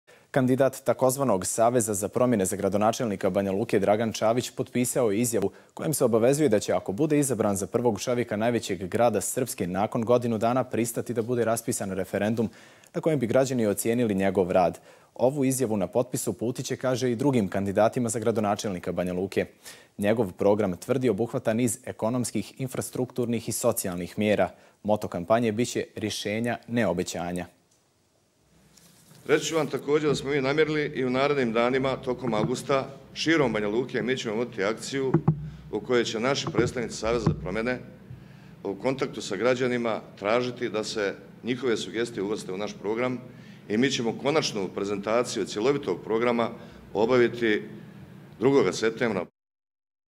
Čavić predstavio izborni slogan
Kandidat opozicionih partija za gradonačelnika Banjaluke Dragan Čavić predstavio je danas izborni slogan ove koalicije pod nazivom "Rješenje, ne obećanje".